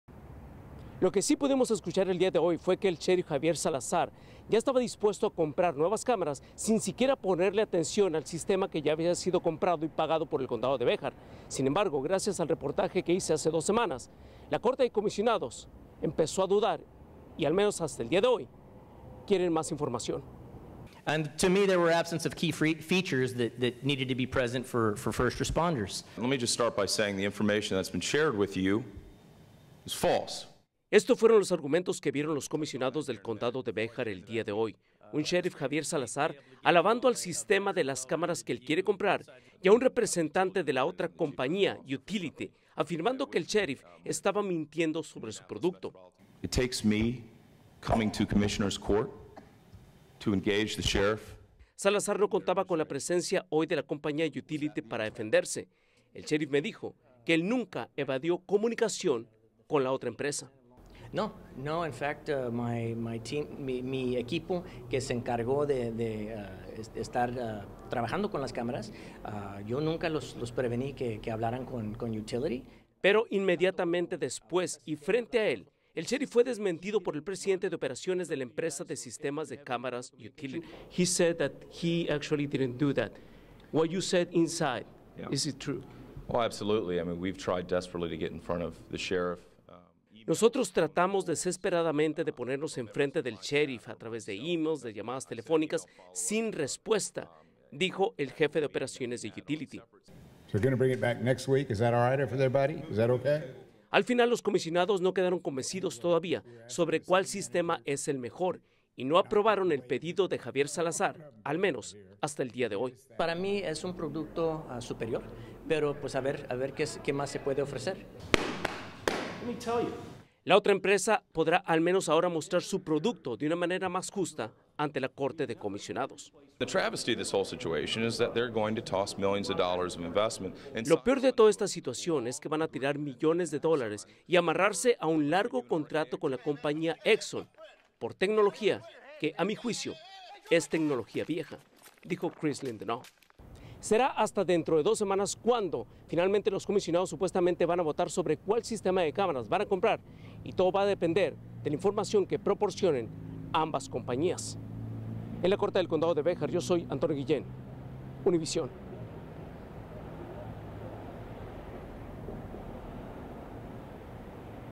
body cams pkg
This is a story that I worked on along with reporter Antonio Guillen. Where he was investigation why the Bexar county sheriff was trying to can the current body cam program for another body cam system that can cost the Bexar county residents a loss of millions of dollars invested in its current system.